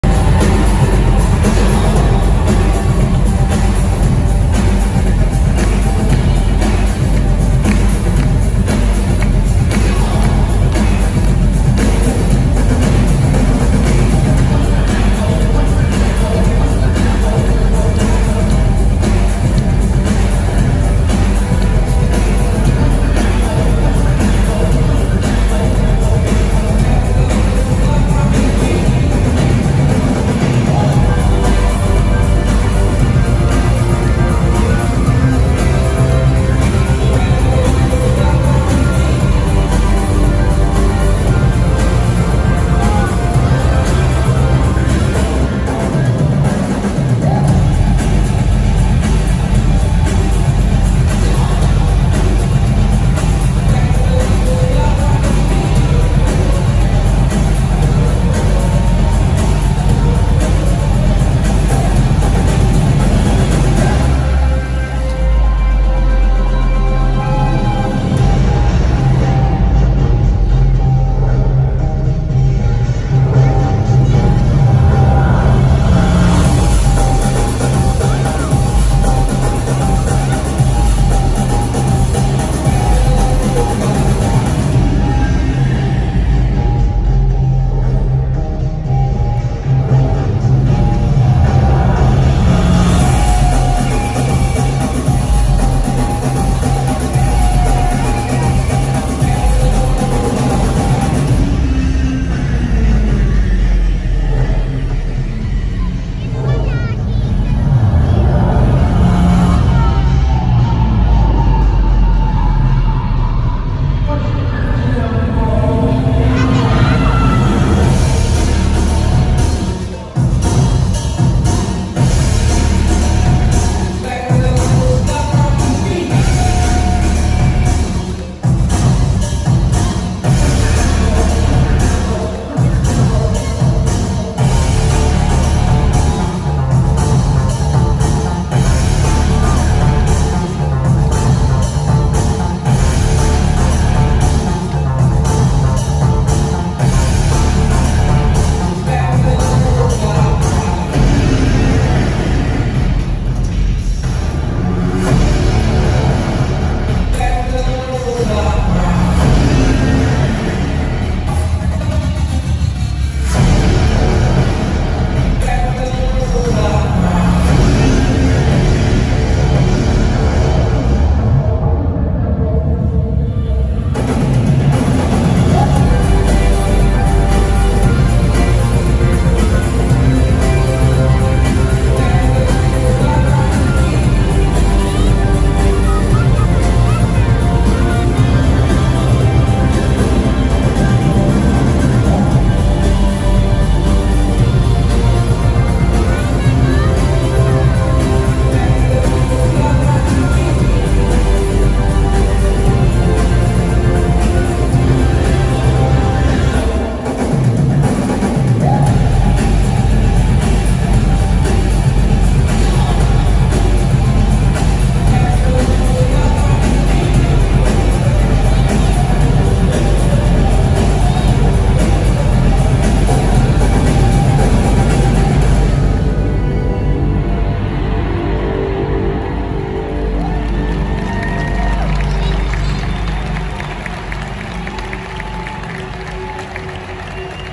inTENse
inTENse  - Aqua Theater Show in Wonder of the Seas
it’s inTENse, the awe-inspiring athleticism and mesmerizing technology are cranked all the way up in this AquaTheater production —only on Wonder of the Seas. This high-energy show brings next-level intensity to everything from pool plunging to tightrope teetering and beyond. Shift all your expectations of what entertainment can be into high gear and prepare for the maximum performance level of inTENse.
This show consists of all amazing Female performers, wow!! So amazing and talented.